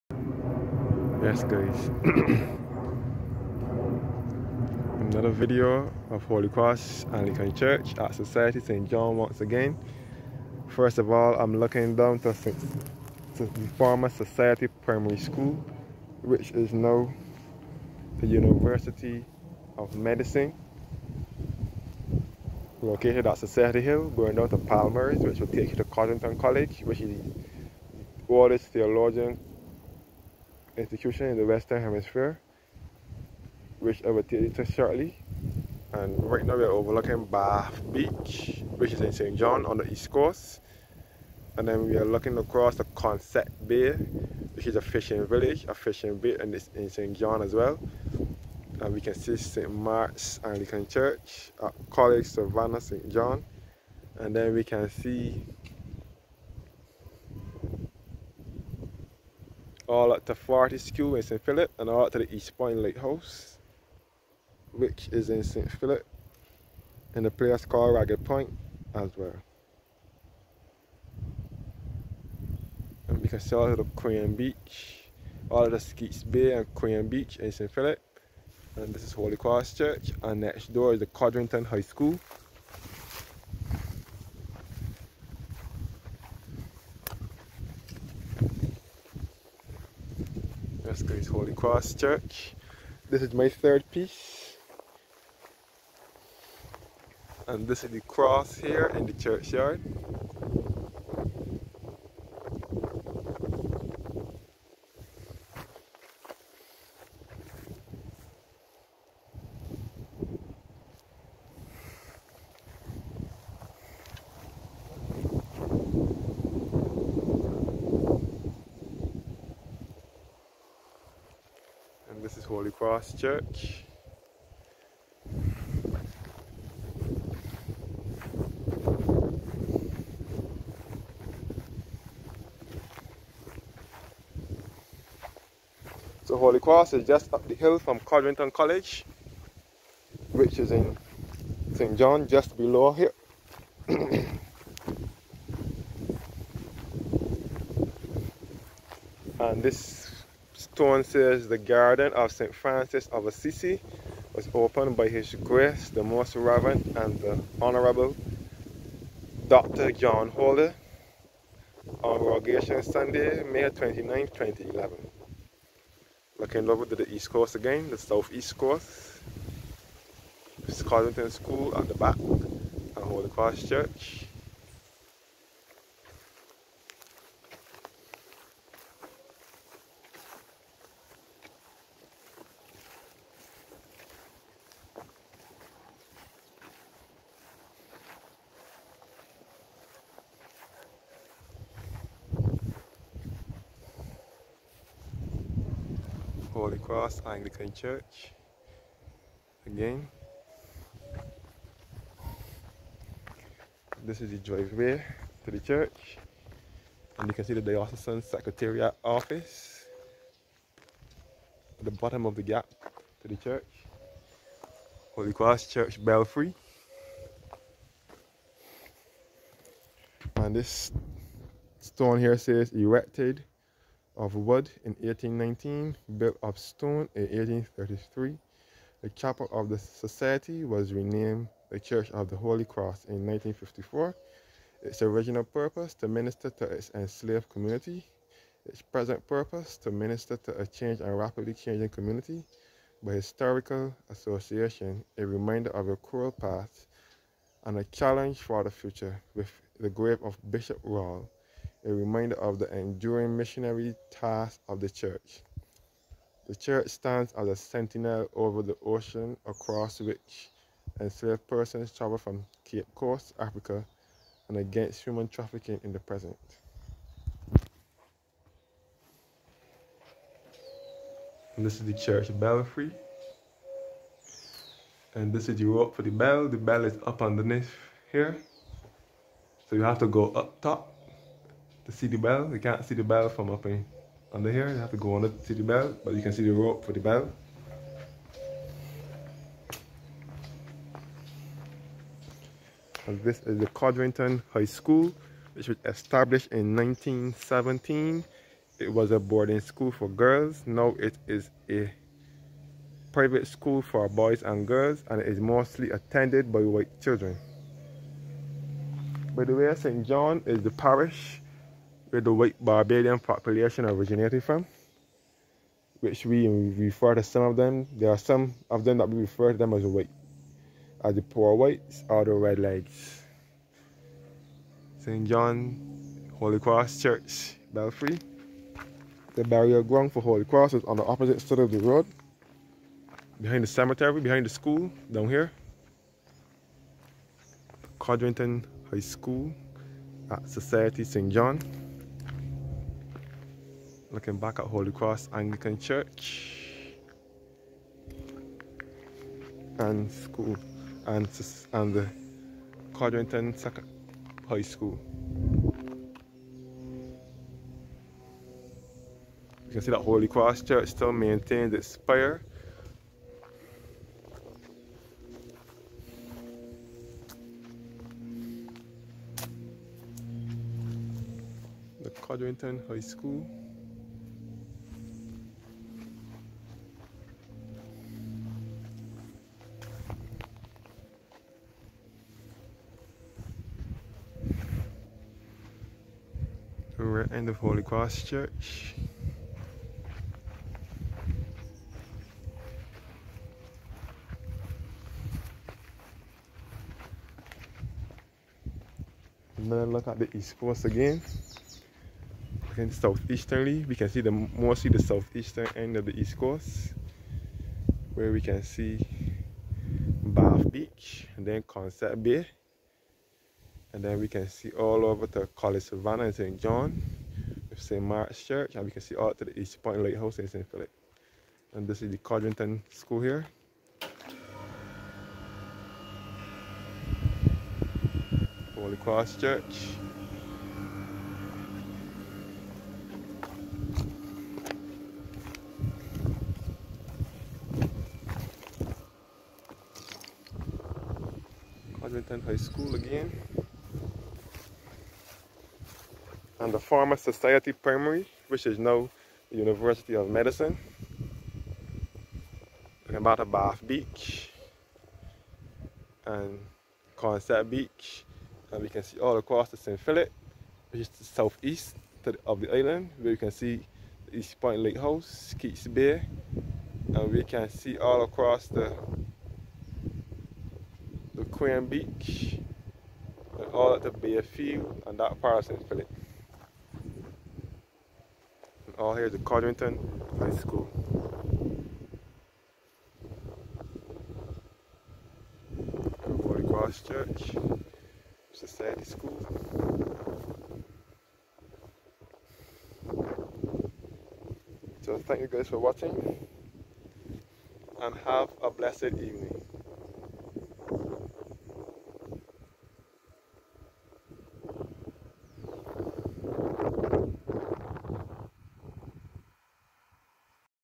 A Walk Around Holy Cross Church St. John Barbados⛪🇧🇧 3
This is a walk around Holy Cross Church in St. John Barbados